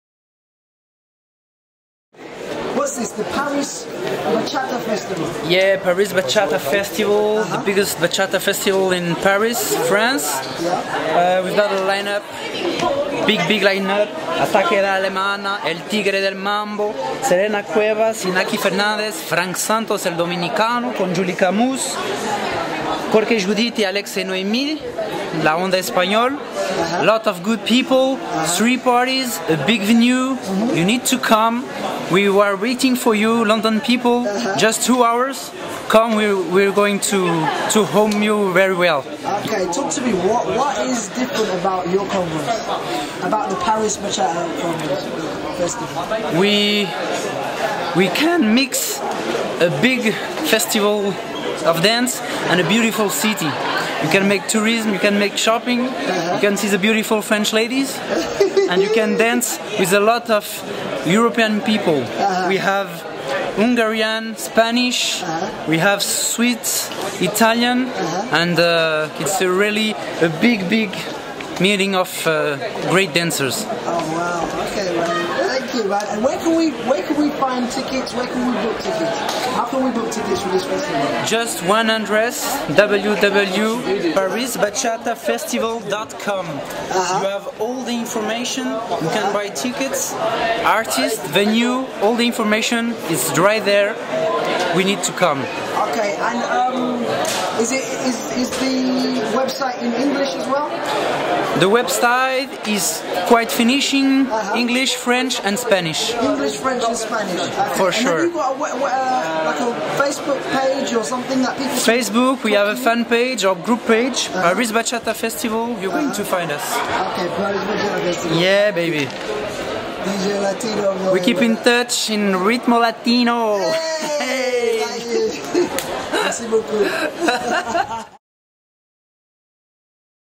Deejay Latinoboy speaks about Paris Bachata Festival 2013
november 21st,22nd,23th &24th 2013